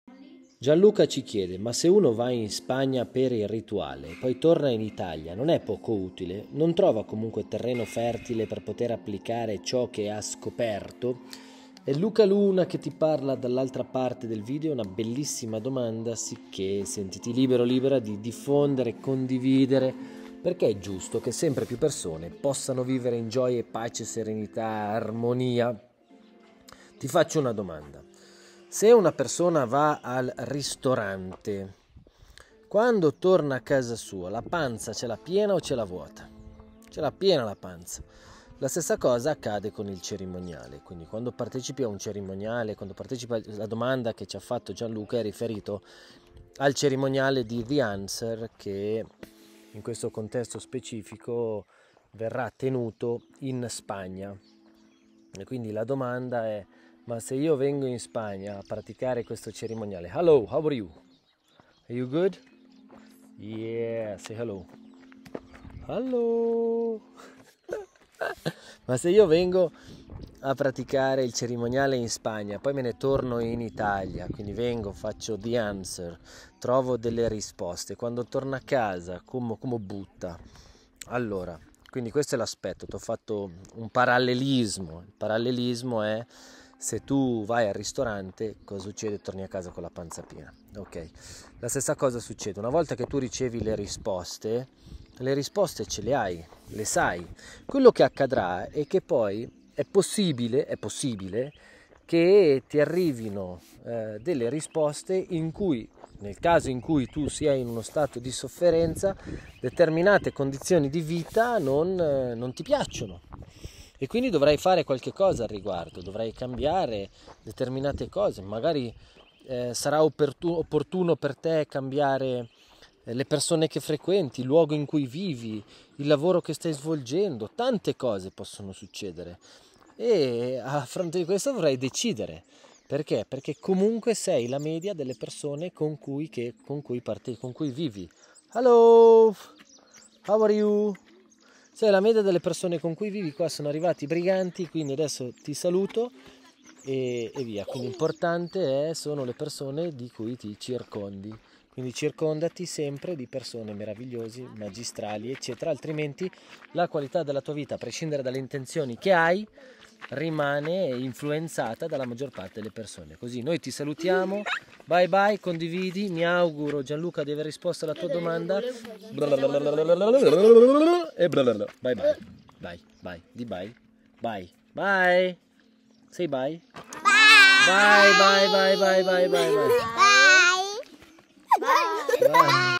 Cerimonie in Spagna e ritorno in Italia, ha senso?
Ma se uno va a fare le cerimonie in Spagna per il rituale e poi torna in Italia, non è poco utile? Una bellissima domanda riguardo le cerimonie, alla quale risponderò con una domanda: se una persona va al ristorante, quando torna a casa sua la pancia ce l’ha piena o vuota?

Se desideri seguire Luka Luna in modo più profondo, connettiti liberamente anche su questi canali: